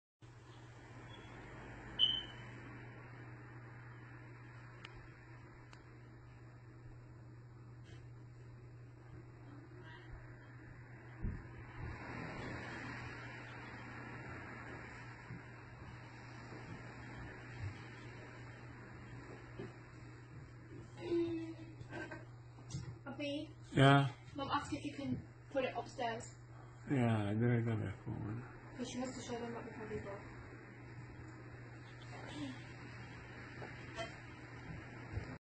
Incidente da paura in Gran Bretagna
Incidente da paura in Gran Bretagna l'auto sportiva arriva a tutta velocità.